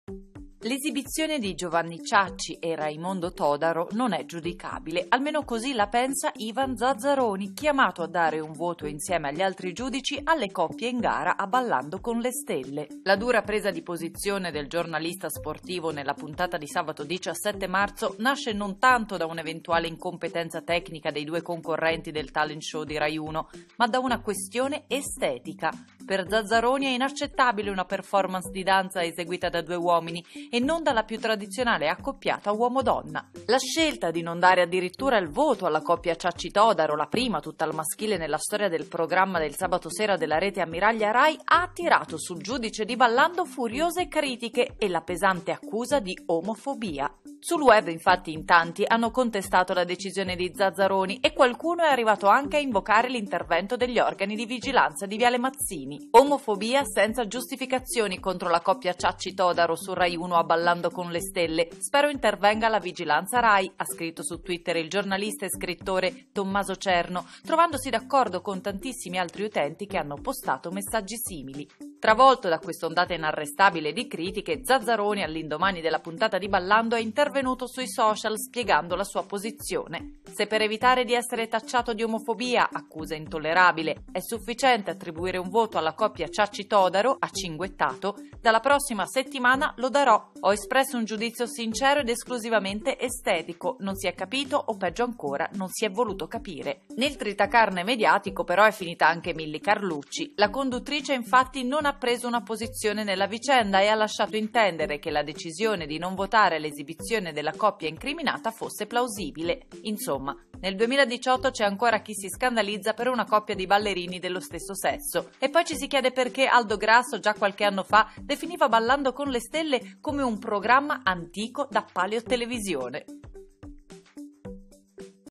'E' inaccettabile', scoppia il caso omofobia a Ballando con le stelle. Verrano presi provvedimenti?
'E' inaccettabile', scoppia il caso omofobia a Ballando con le stelle. Verranno presi provvedimenti?
Scintille nella seconda puntata di Ballando con le Stelle 2018. Hanno scatenato un putiferio le parole che Ivan Zazzaroni ha riservato a Giovanni Ciacci e Raimondo Todaro, la prima coppia di ballerini nella storia del talent show di Rai Uno formata da persone dello stesso sesso. Prima la reazione del pubblico in studio, poi contro il giudice anche l'indignazione sui social dove in tanti chiedono addirittura l'intervento della vigilanza Rai. Il giornalista sportivo ha replicato su Twitter: basterà un post per calmare le acque o verranno presi provvedimenti? Ecco cosa è successo nel programma di Milly Carlucci